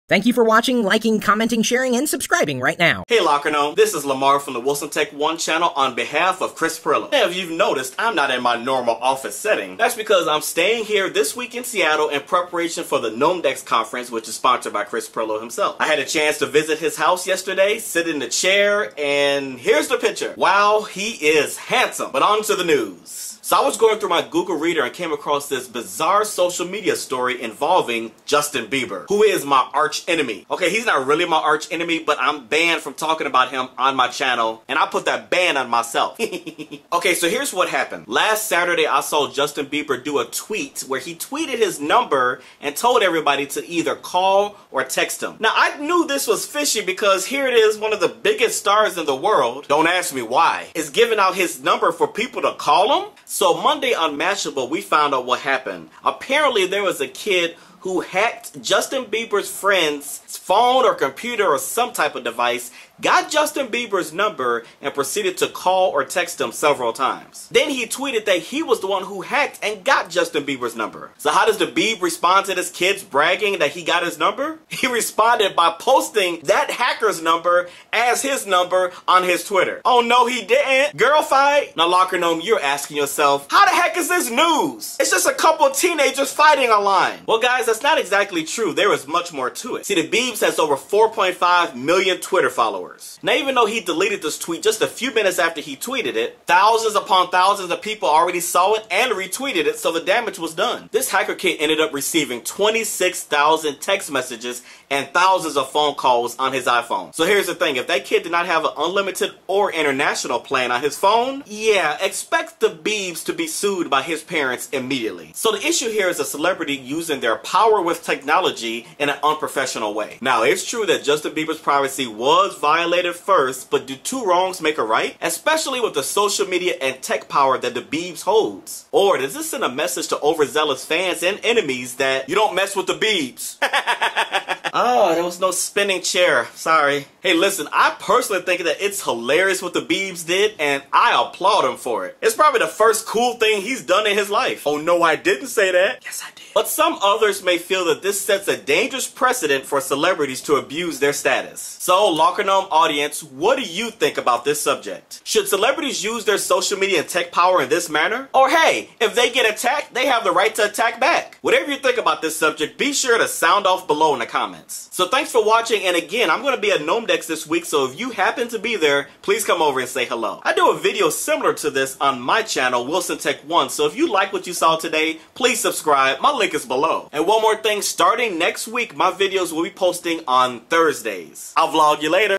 Did Justin Bieber Abuse his Social Media Power?
- This week, Lamarr is all worked up over Justin Bieber and his apparent mis-use of Twitter. It seems as though JB was upset after a fan hacked the computer of a friend of the Bieb to obtain Justin's cell phone number. This "hacker" then called and texted JB a few times. In order to get back at the poor kid, Justin tweeted HIS phone number out, claiming it was his own. The kid received thousands upon thousands of text messages and telephone calls. To me, this is a blatant case of abusing one's power, and JB owes this kid a public apology - and then some. Lamarr doesn't necessarily agree, though. What do YOU think?